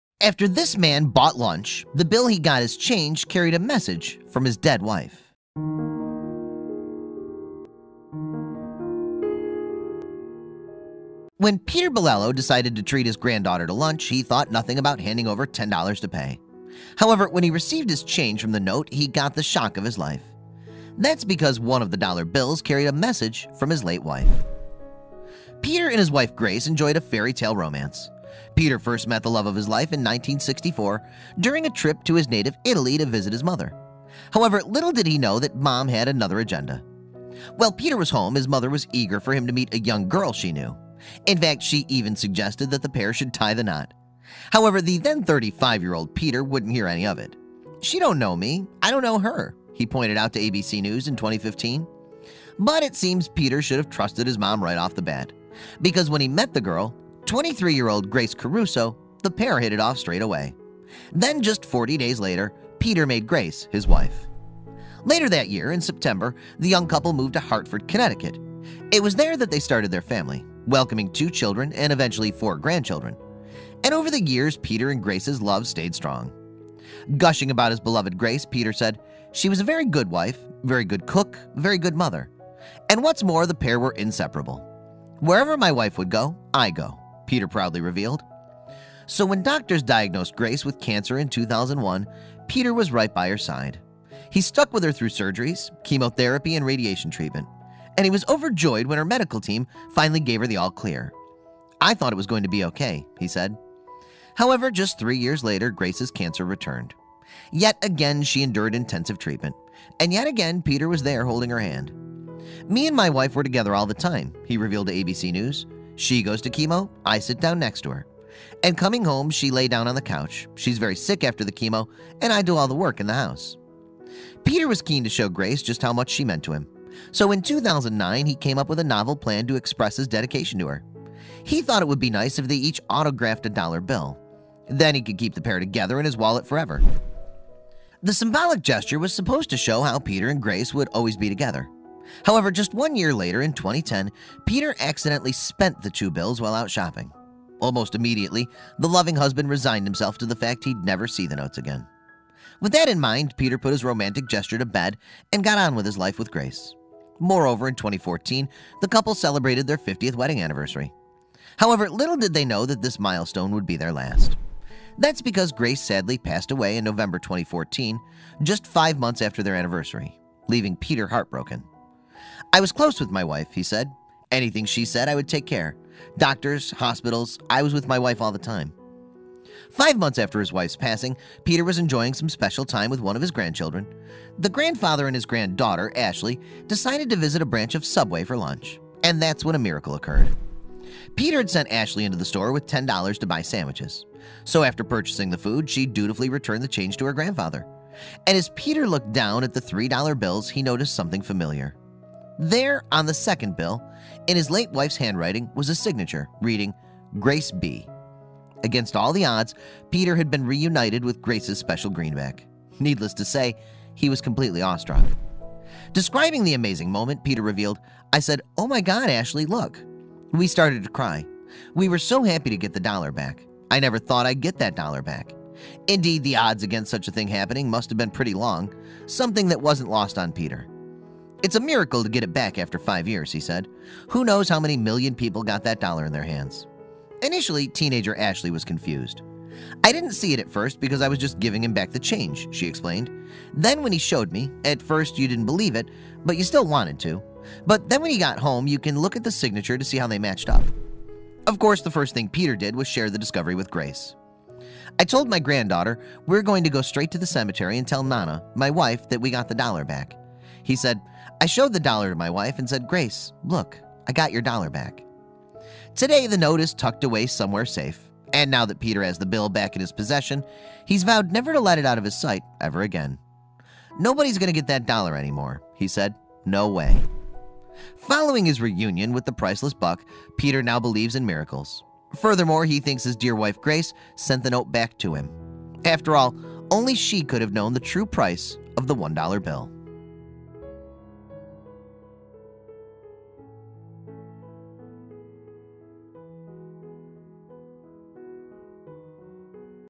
After This Man Bought Lunch, The Bill He Got As Change Carried A Message From His Dead Wife
When Peter Bilello decided to treat his granddaughter to lunch, he thought nothing about handing over $10 to pay. However, when he received his change from the note, he got the shock of his life. That’s because one of the dollar bills carried a message from his late wife.